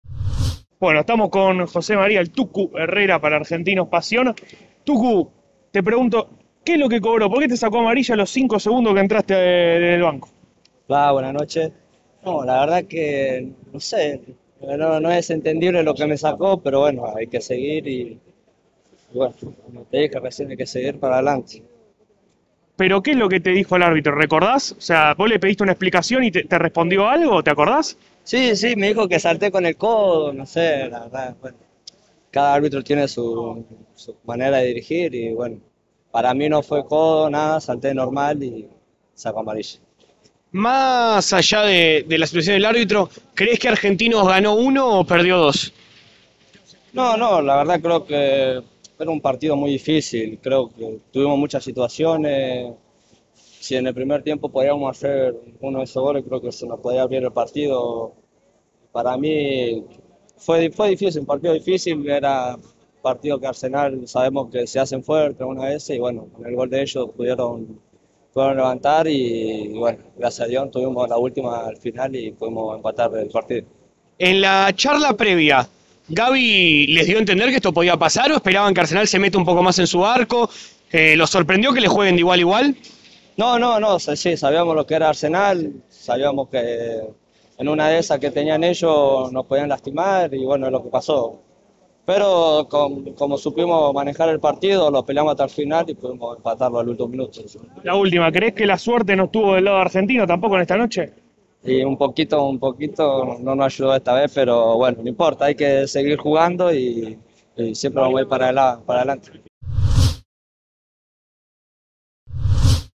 AAAJ 1-1 ARSENAL // Tucu Herrera: "Sabíamos que iba a ser un rival complicado"
Muchas gracias!